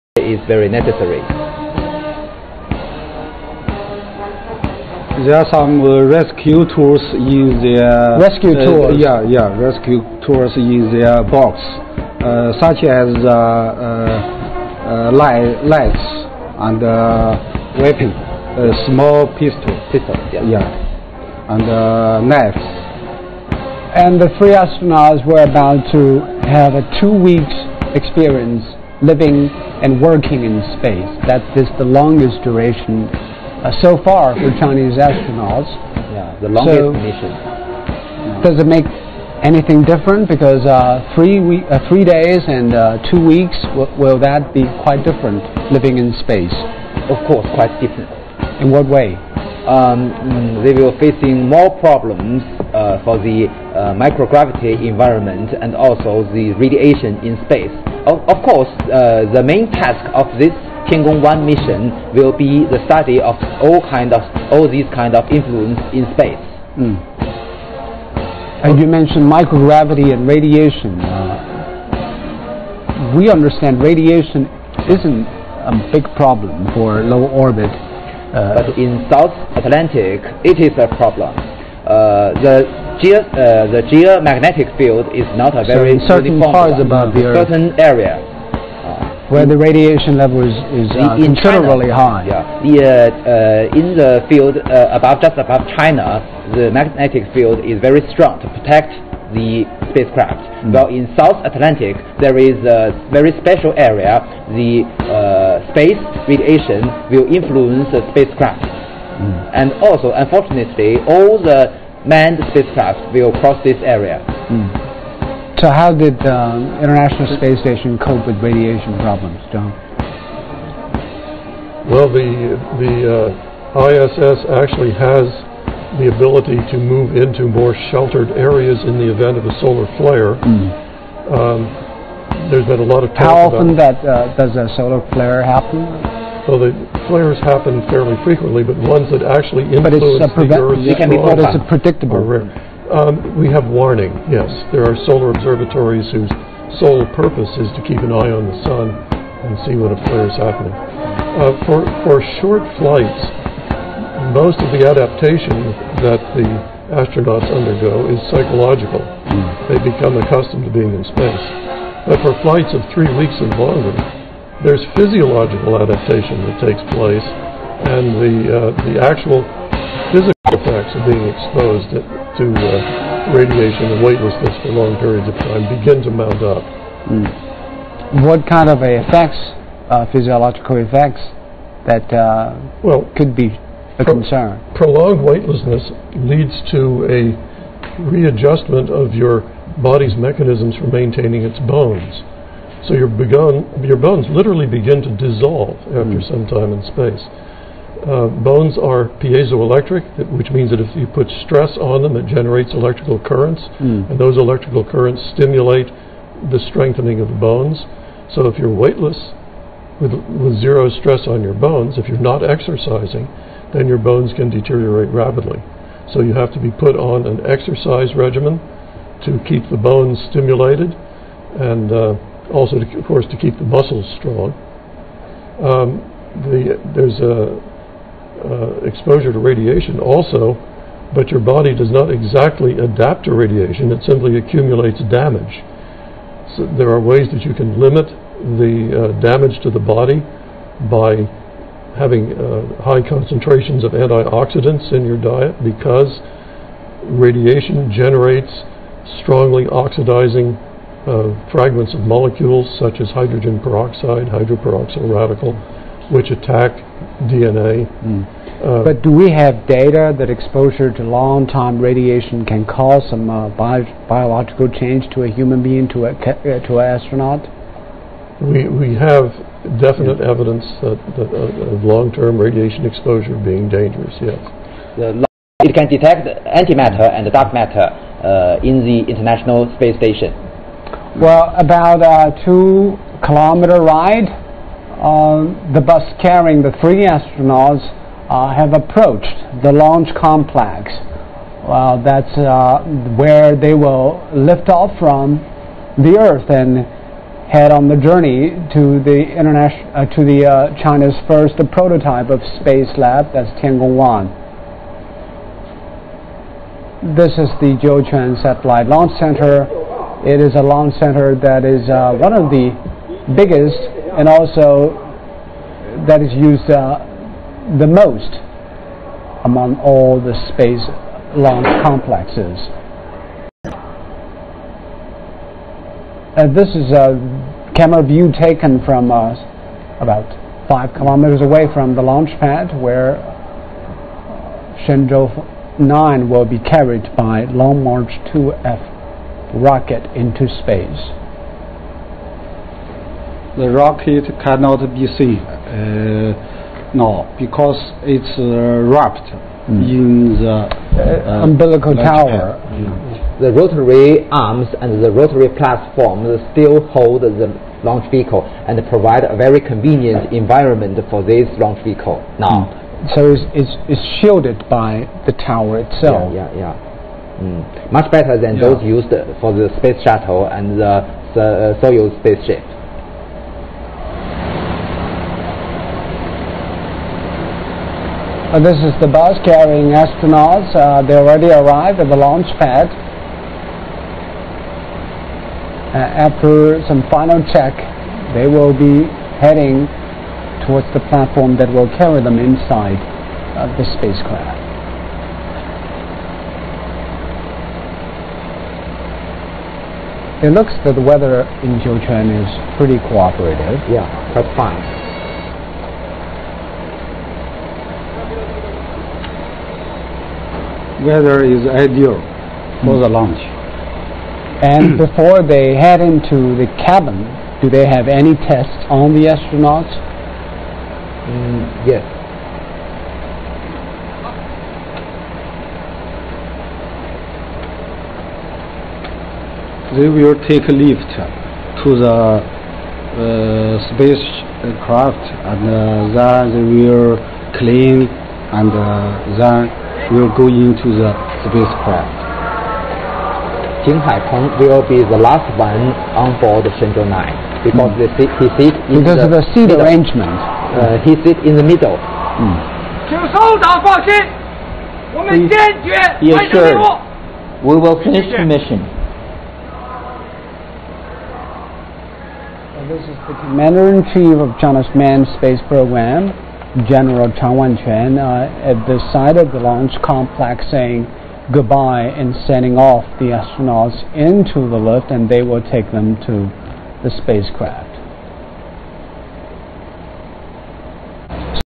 Shenzhou 9 Astronauts Send Off-1 06-18-2012
Sending astronauts off to launch platform.